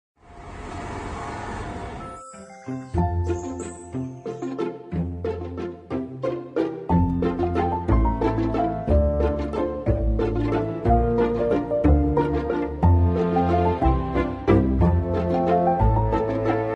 Mommy Grey the Silver Grey Golden Hamster 🐹
6 healthy Babies of Mommy Grey 🐹🐹🐹🐹🐹🐹